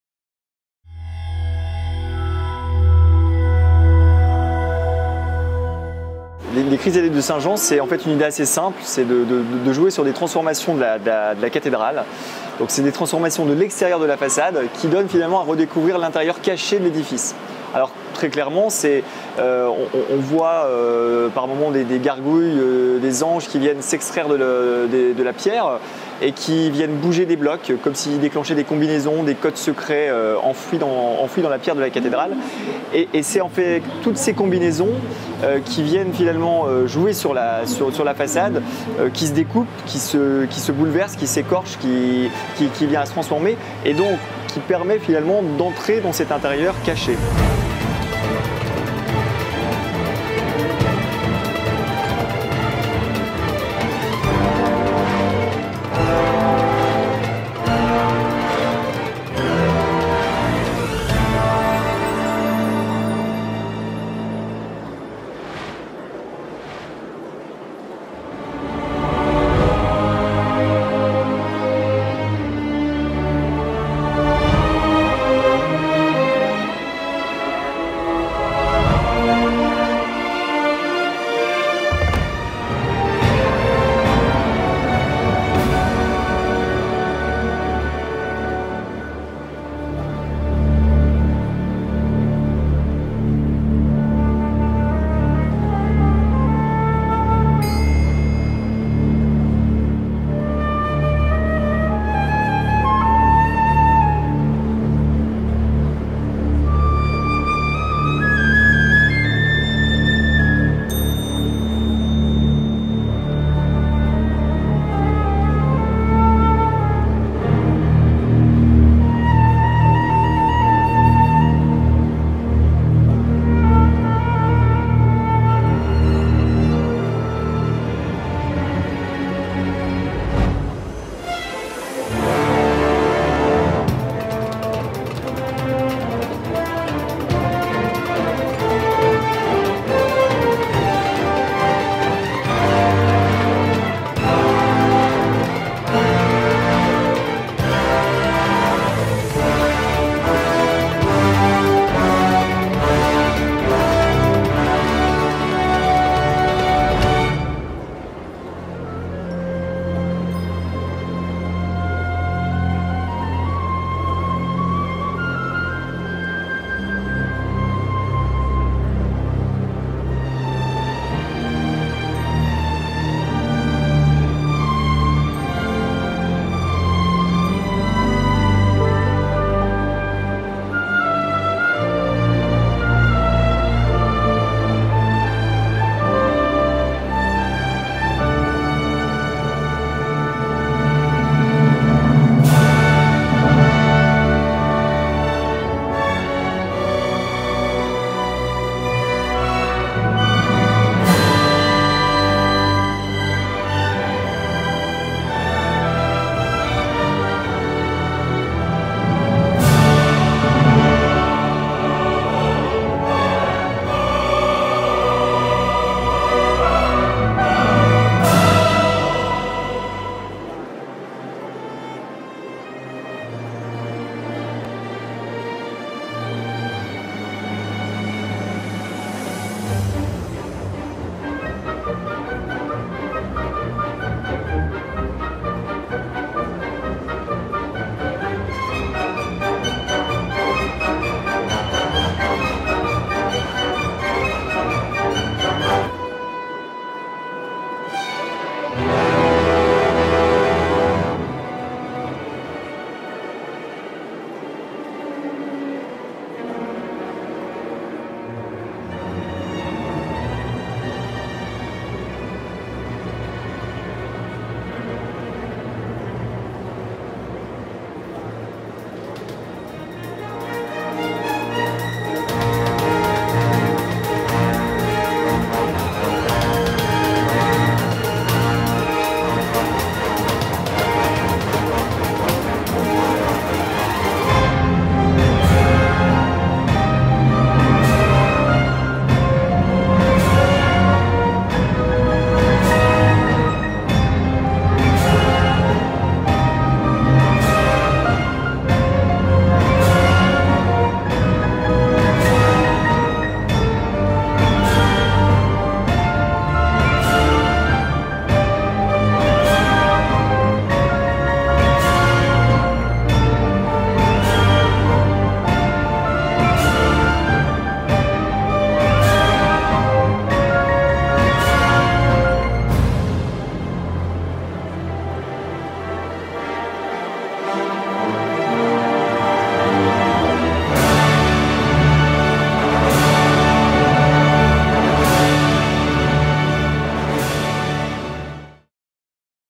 Les Chrysalides de Saint Jean, Fête des Lumières (Lyon) - Décembre 2012
Vainqueur du Trophée des Partenaires 2012 !

Les Chrysalides de Saint Jean, Fête des Lumières (Lyon) - Décembre 2012, une création de Damien Fontaine pour La Maison Production.